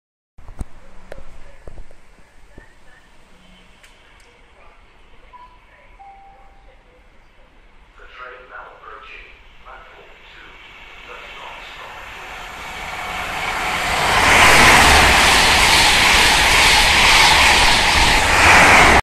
FGW Class 43 Passing Maidenhead
Class 43 Train passing though to Maidenhead heading towards Reading.

No point putting any sort of copyright on this if your gonna nick it then i cant stop you ;-) but at least give me credit if you do :-)